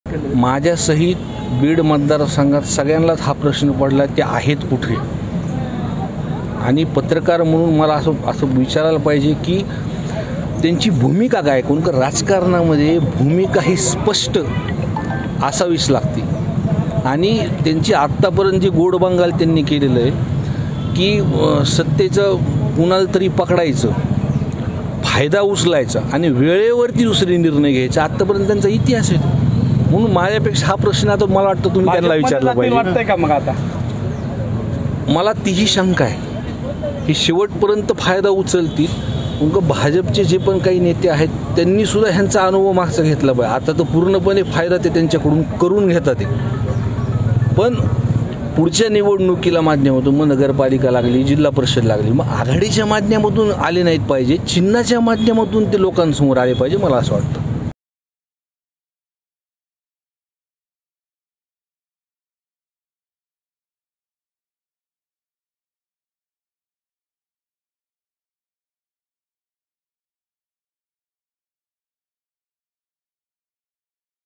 Sandip Kshirsagar Vs Jaidatta Kshirsagar : हिंमत असेल तर चिन्ह घेऊन समोर या... | LetsUpp Marathi
बीडमधील क्षीरसागर काका पुतण्याचा वाद चिन्हांवरून पुन्हा एकदा चव्हाट्यावर आला आहे. आमदार संदीप क्षीरसागर यांनी आपले काका जयदत्त क्षीरसागर यांना इकडे तिकडे न करता निवडणुकीसाठी चिन्ह घेऊन समोर या असे खुले आव्हान दिले आहे.